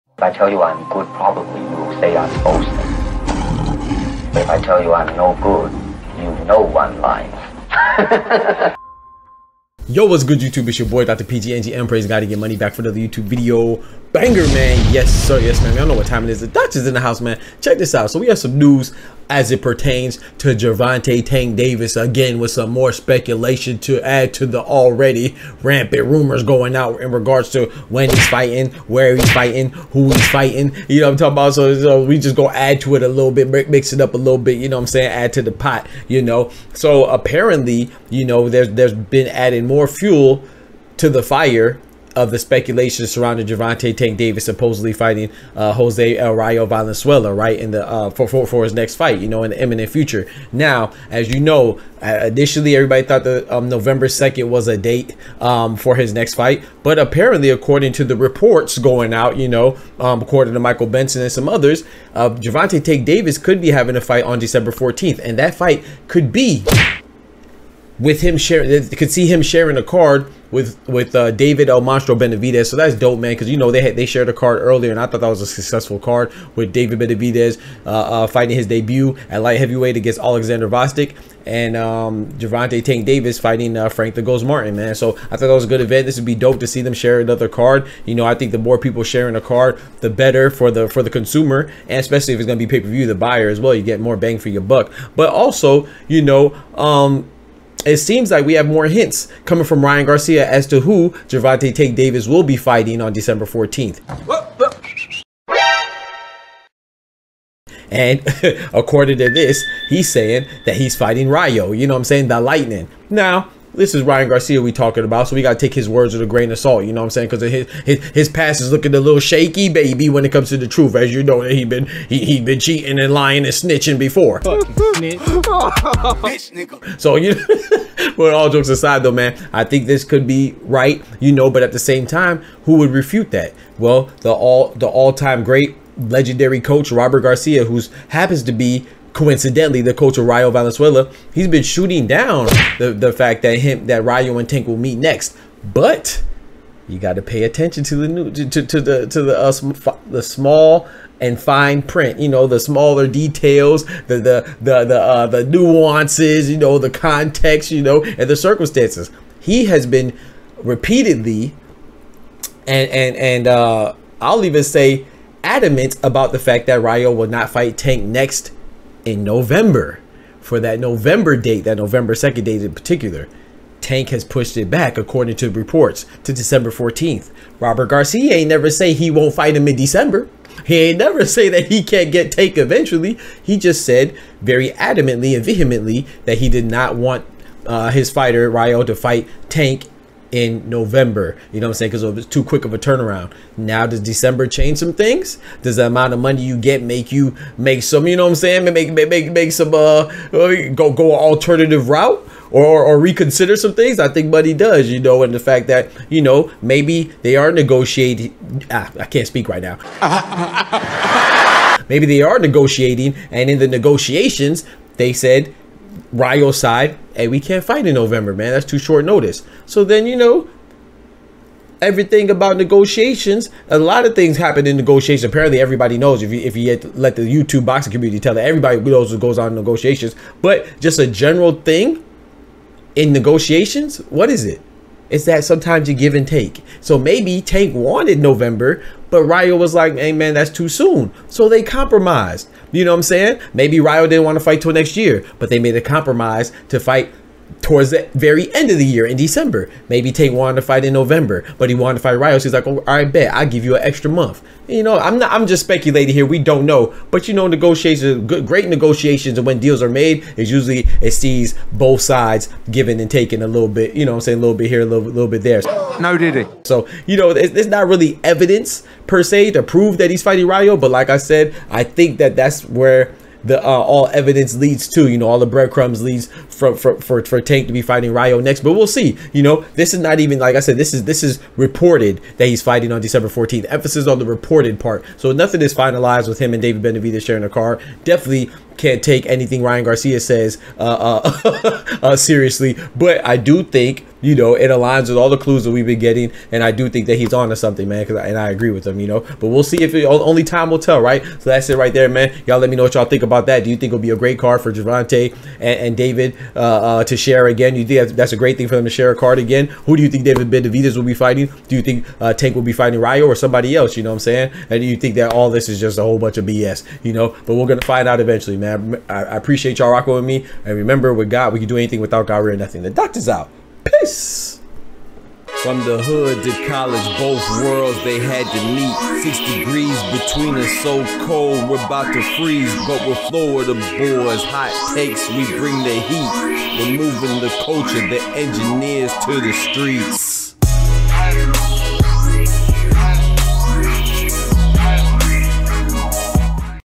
RYAN GARCIA SPILLS THE BEANS, EXPOSES GERVONTA DAVIS' NEXT OPPONENT | Is Ryan Right?
Gervonta Davis has a lot of rumors circulating about his next fight, nobody knows who it will be...but did Ryan Garcia just expose Tank's next fight? It could be with David Benavidez on the same card. Doc discusses!